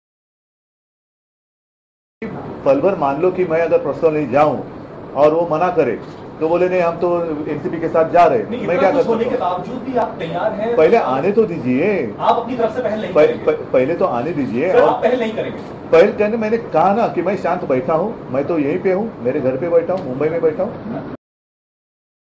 Let them make the first move: Uddhav Thackeray
Shiv Sena president Uddhav Thackeray made it clear his party will not lend unsolicited support to BJP in forming the government in Maharashtra, saying "let them make the first move".